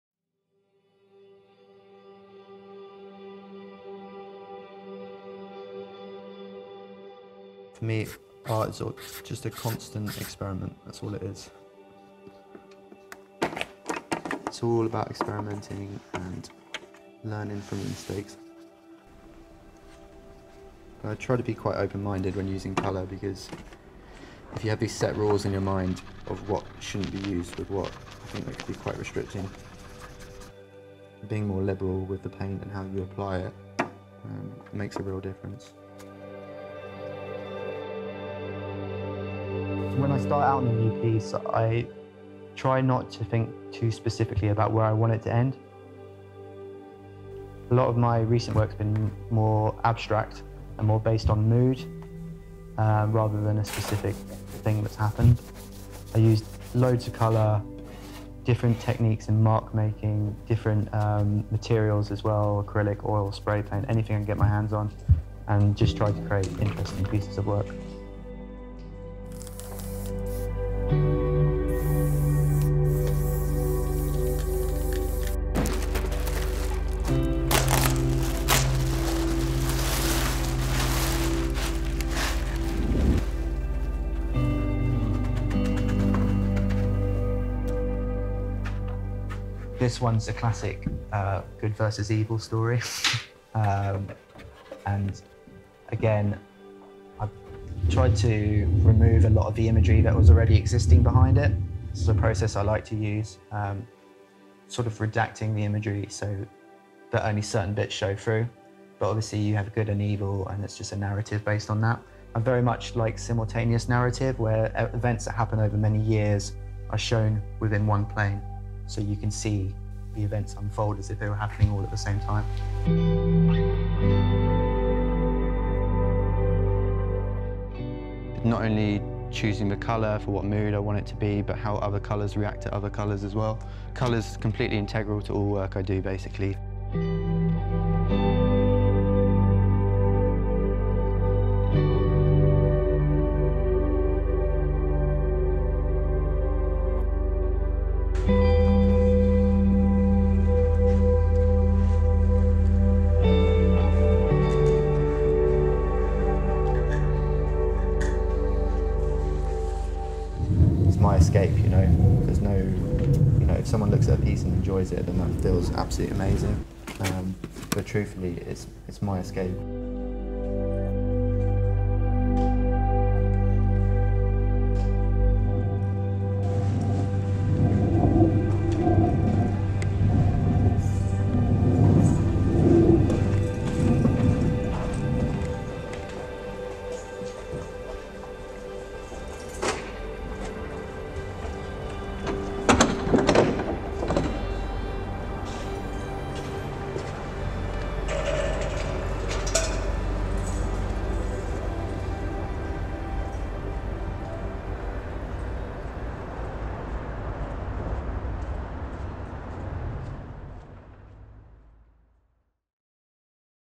Color Diaries | Episode 1
Color Diaries is a series of short documentaries on creatives working interestingly with color. It's our way to celebrate our love for color and people who share this love with us.